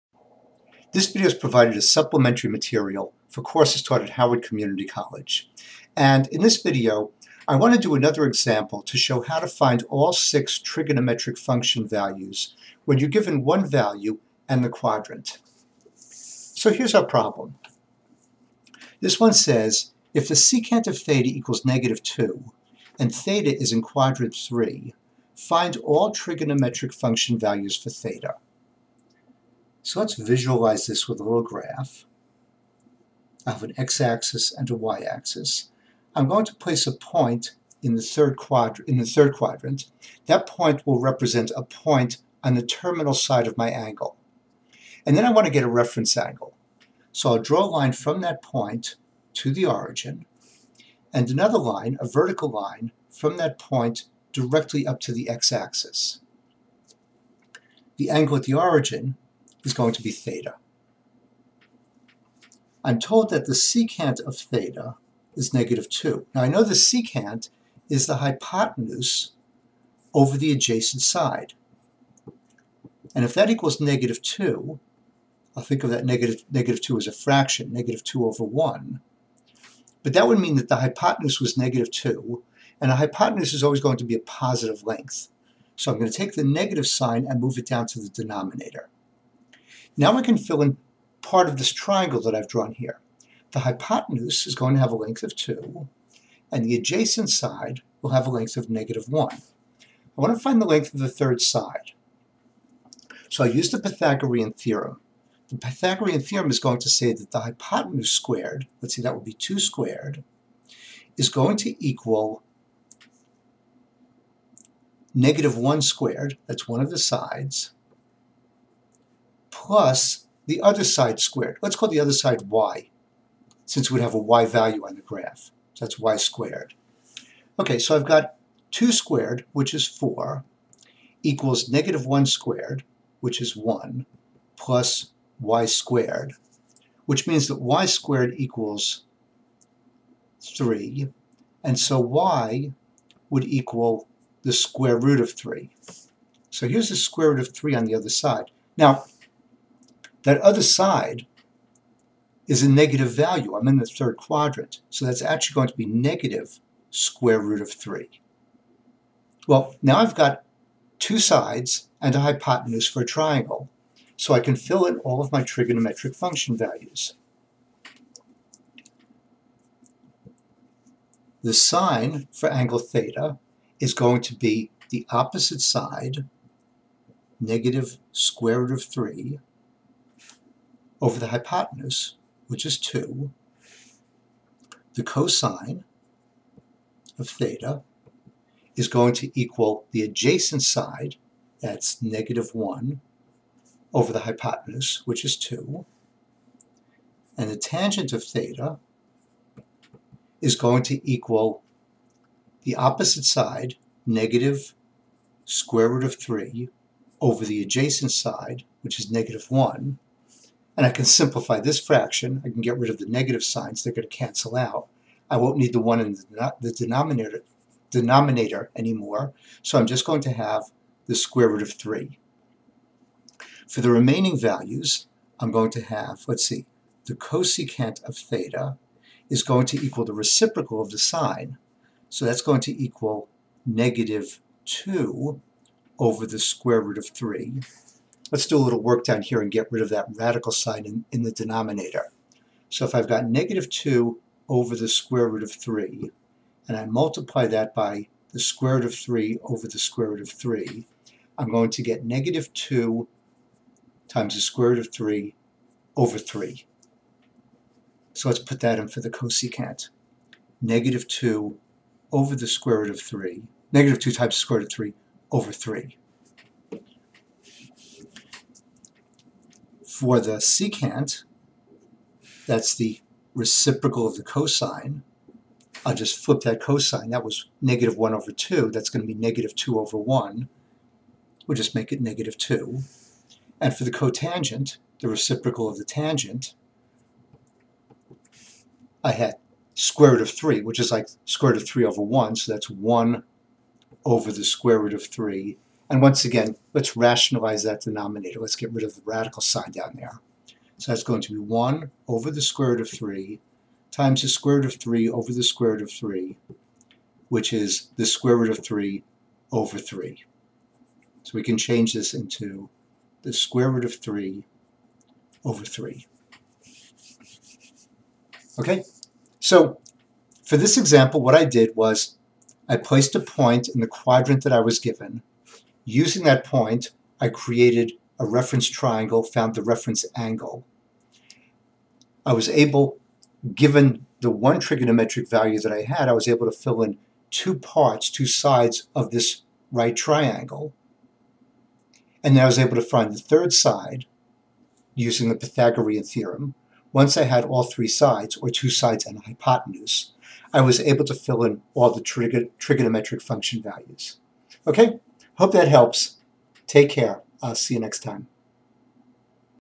Finding Trig Function Values Given One Value and the Quadrant, another example 143-8.5.4.b
How to find all six trigonometric function values given one value (the secant) and the quadrant. This video is provided by the Learning Assistance Center of Howard Community College. For more math videos and exercises, go to HCCMathHelp.com.